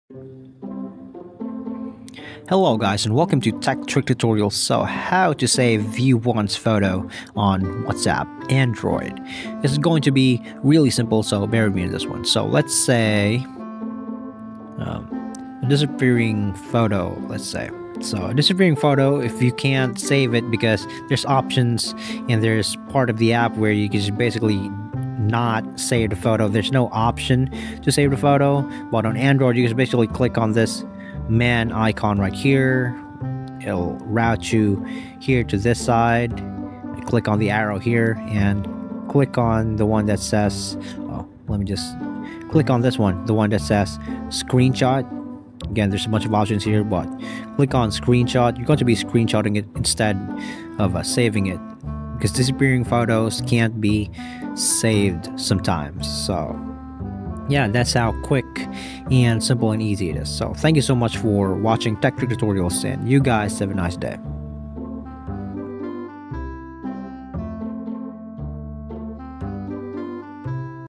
How to Save View Once Photo on WhatsApp Android (Quick Tutorial)
Learn how to save a view-once photo on WhatsApp for Android with this quick tutorial! 📸📱 Follow these simple steps to keep the image before it disappears. 🚀✨